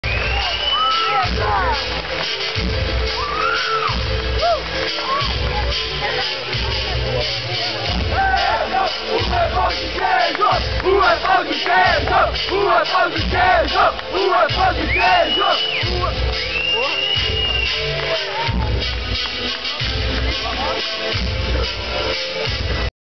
Olimpíada do Conhecimento - CAD 2° Lugar
Eu na premiação da Olimpíada do Conhecimento 2008 - Etapa Nacional Blumenau - SC